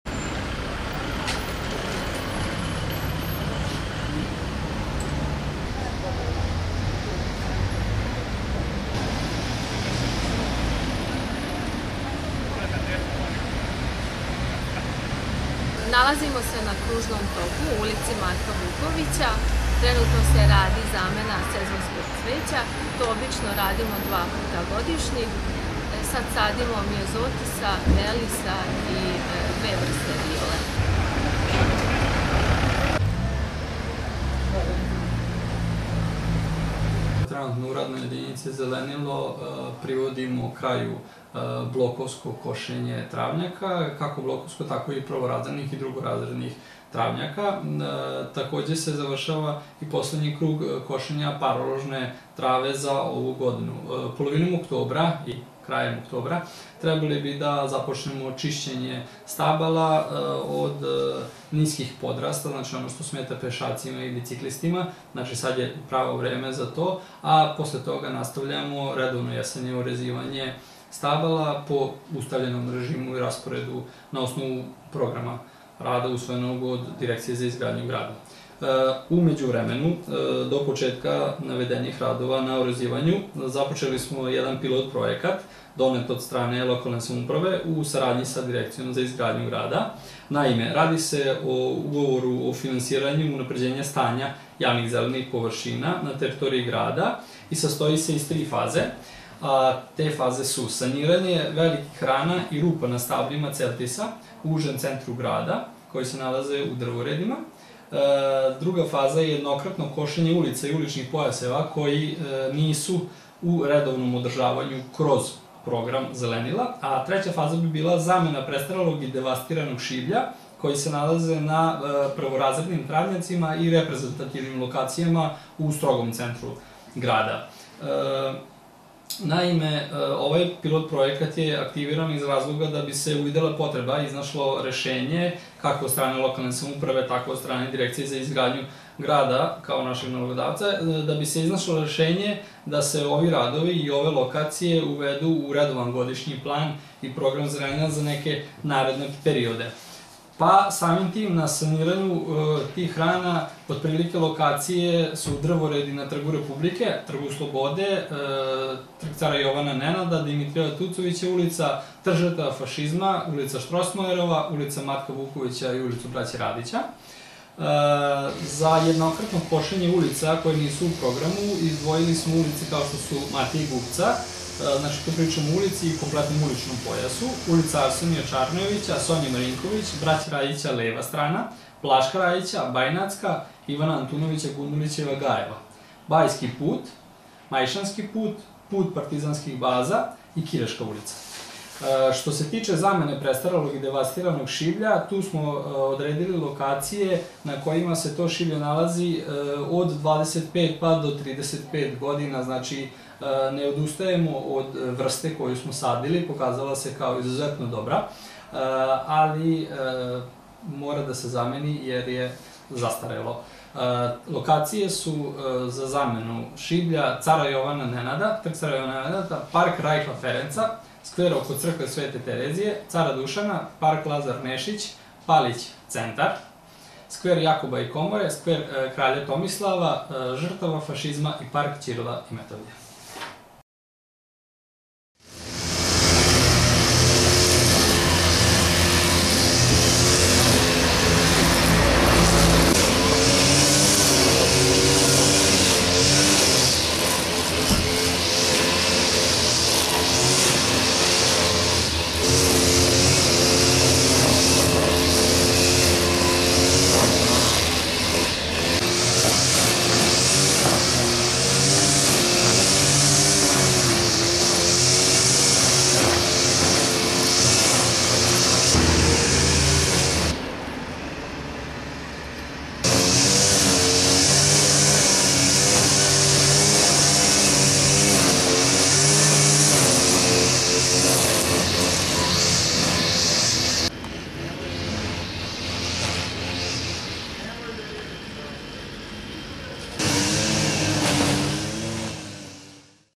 Aktivnosti RJ Zelenilo 28092016
super-info.rs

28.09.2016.

Očišćen deo divlje deponije u Donjem Tavankutu

Jesenja akcija čišćenja i uređenja Grada Subotice započeće 3. oktobra i trajaće do 10. decembra, kako je najavljeno u JKP "Čistoća i zelenilo" Subotica. Trenutne aktivnosti u ovom javno - komunalnom preduzeću, odvijaju se na sanaciji dela divlje deponije u Donjem Tavankutu, zameni sezonskog cveća na kružnom toku u ulici Matka Vukovića, a privodi se kraju i blokovsko košenje travnjaka i parložnih trava.

Divlja deponija u Donjem Tavankutu je preko dve decenije odlagalište otpada stanovnika iz mesnih zajednica Donjeg i Gornjeg Tavankuta, Male Bosne i Ljutova. Po rečima organizatora u Radnoj jedinici Higijena grada, Jože Červenija, tokom protekle tri nedelje, u prvoj fazi čišćenja deponije uklonjeno je oko 1.500 tona otpada koji je sasvim blokirao prolaz kroz ulice Nikole Tesle i Ulice Malina.
- Kratak je bio rok, ali smo uspeli da sa angažovanjem većeg broja kamiona odnesemo ovako ogromnu količinu smeća i obezbedimo prohodnost puteva, kaže Jožo Červeni.
Radovi na čišćenju divlje deponije u Donjem Tavankutu finansirani su iz gradskog budžeta, preko Sekretarijata za poljoprivredu i zaštitu životne sredine, sa 2.5 miliona dinara.

Po rečima rukovodioca RJ Zelenilo, Dejana Petriča, ova radna jedinica u okviru JKP "Čistoća i zelenilo" Subotica, polovinom oktobra će započeti čišćenje stabala od niskih podrasta, a nakon toga sledi redovno jesenje orezivanje stabala i nastavak realizacije pilot projekta unapređenja stanja javnih, zelenih površina na teritoriji grada.

- JKP "Čistoća i zelenilo" Subotica je započelo sa realizacijom pilot projekat unapređenja stanja javnih, zelenih površina na teritoriji grada, u okviru kog će se vršiti sanacija i zaceljenje rana i rupa na stablima celtisa na području užeg gradskog jezgra, jednokratno košenje ulica i uličnih pojaseva koji do sada nisu bili obuhvaćeni programom redovnog održavanja i zamena prestarelog i devastiranog šiblja, kaže Dejan Petrić.